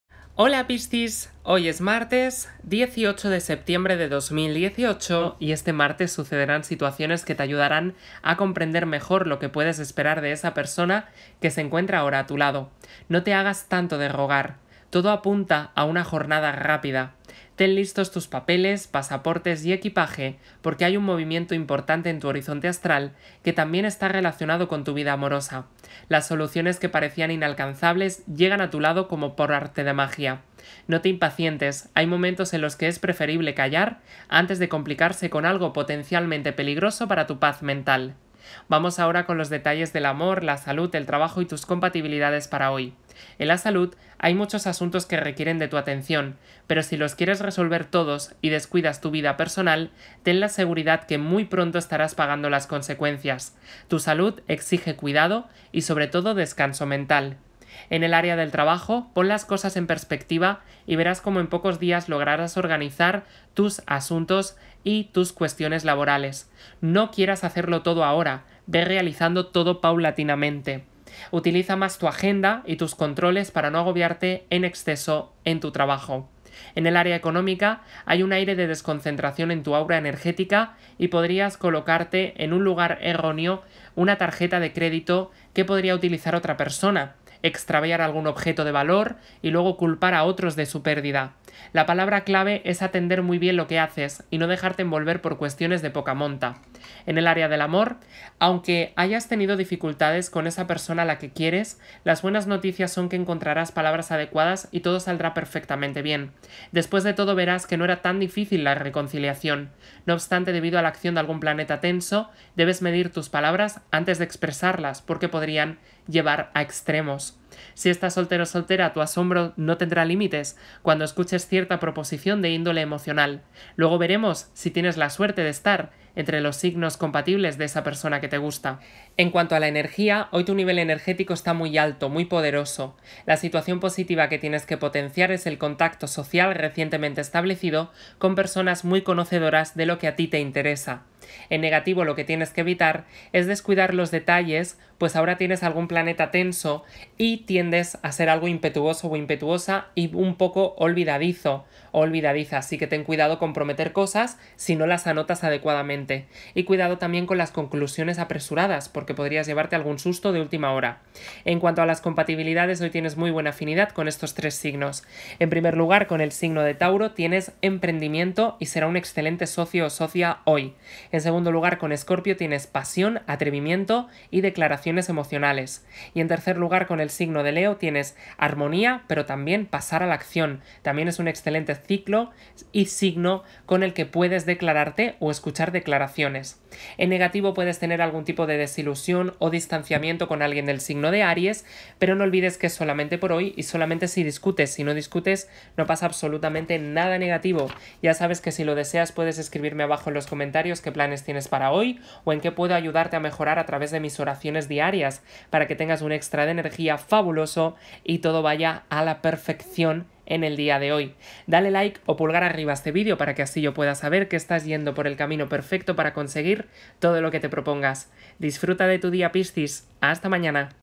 Horóscopo Diario - Piscis - 18 de Septiembre de 2018
Horóscopo de hoy piscis 18 de Septiembre de 2018. Horóscopo de piscis hoy. Descubre tu futuro con mi horóscopo diario para ti, mi querido signo piscis en el amor, el trabajo, la salud, el dinero y las compatibilidades con otros signos. Horóscopo diario gratis!